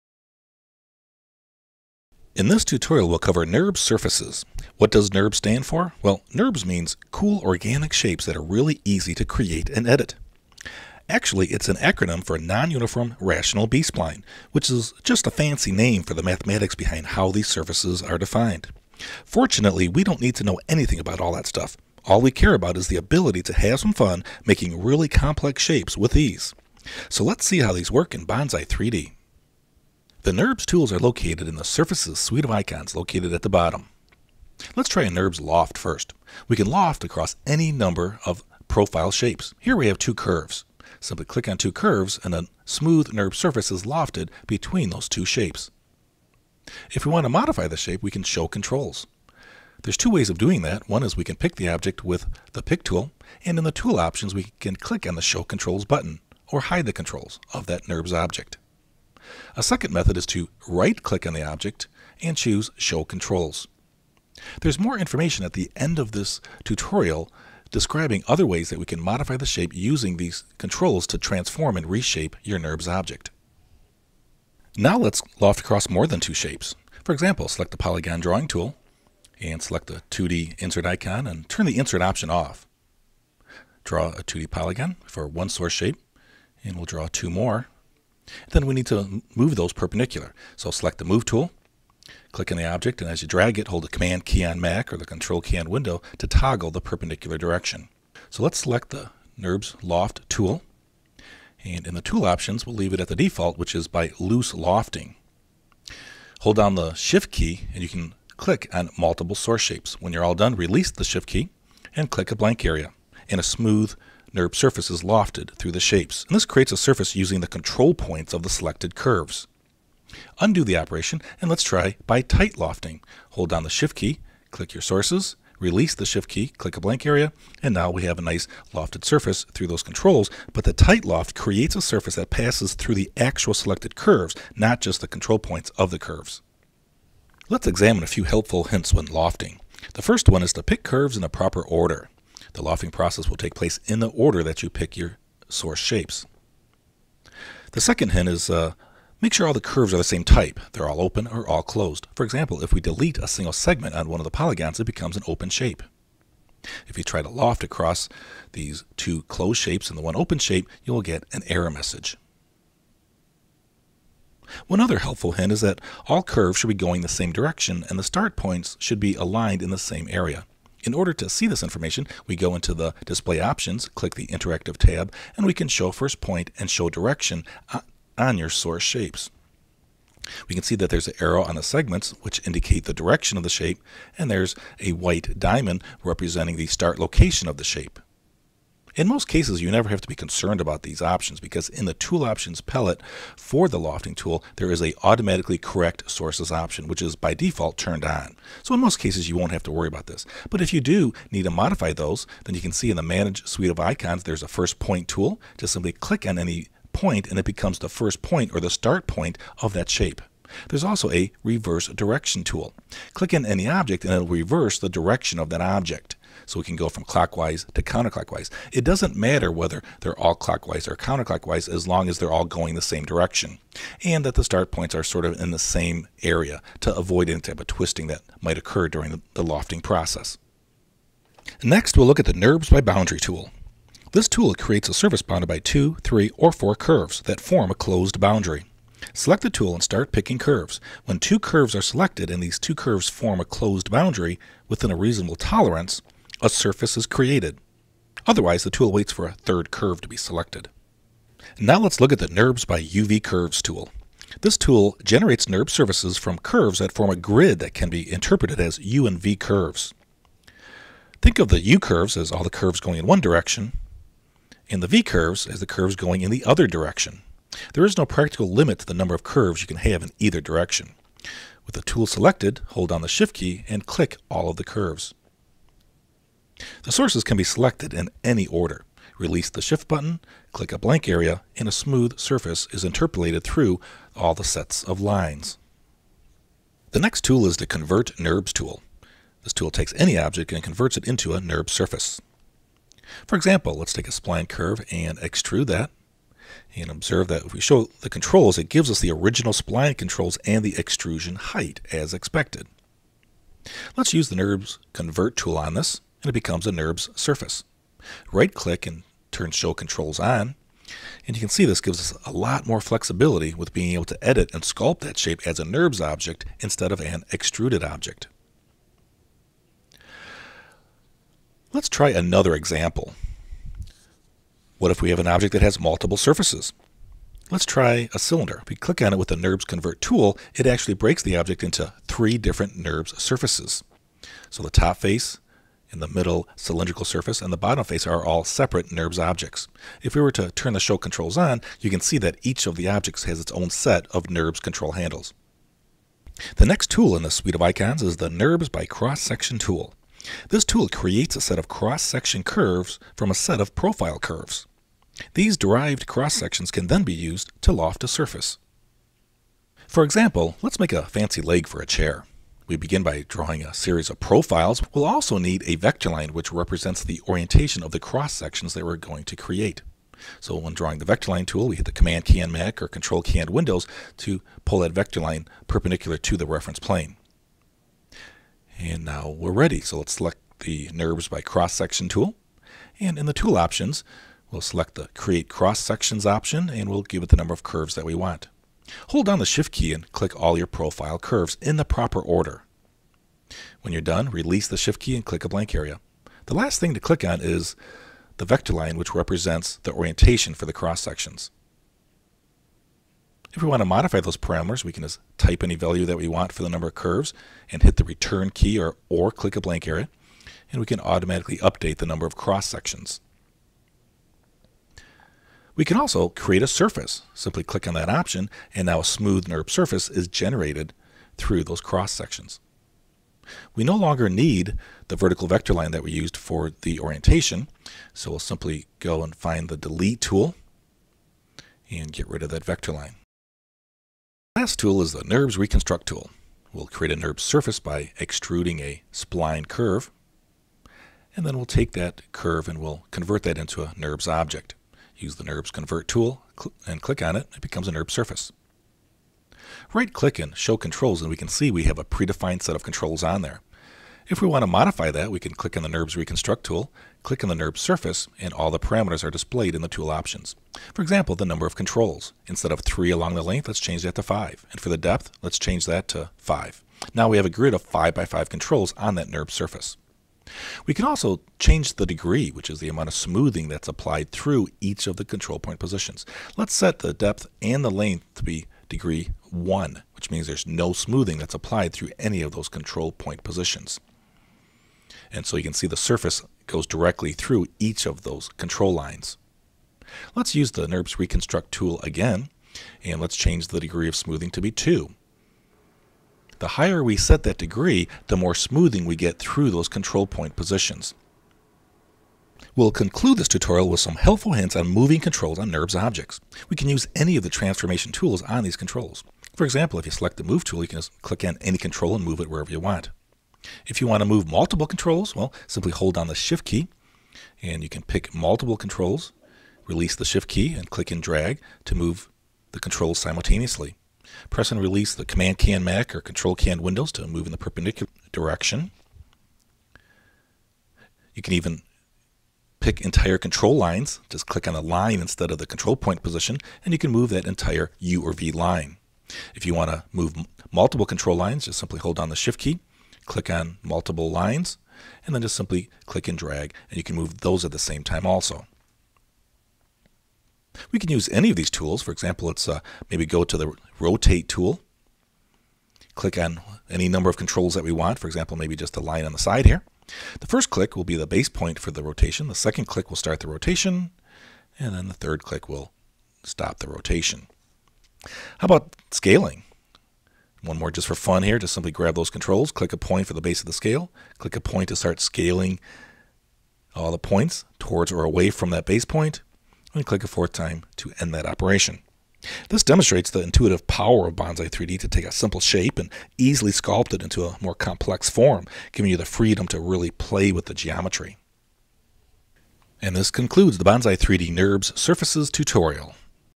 bonzai3d Nurbs Surfaces
Create, edit and sculpt complex forms using the nurbs tools in bonzai3d. Well also see how other objects can be converted to nurbs objects. By Matthew Holewinski.